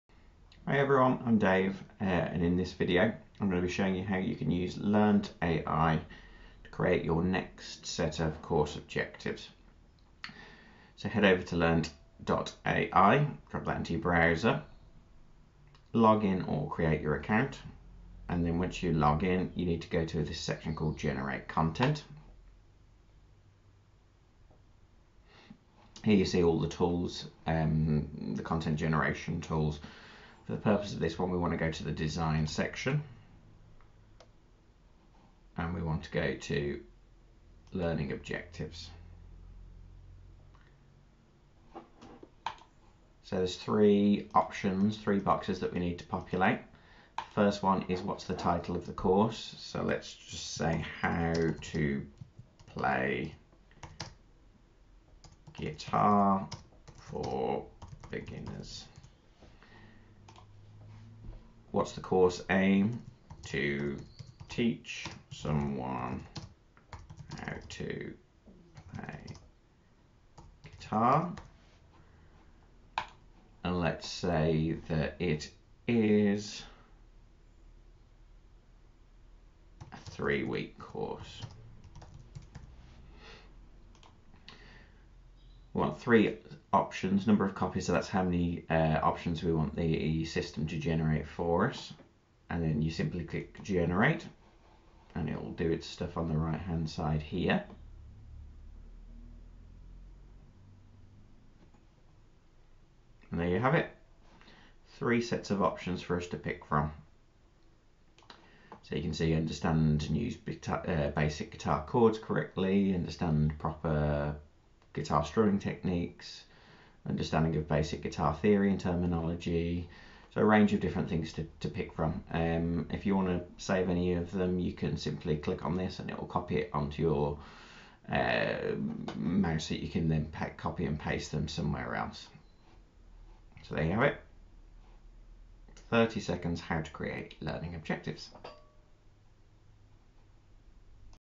How to write Learning Objectives in Seconds with Learnt.ai
Find out how you can create learning objectives for your next course in seconds, with the power of AI.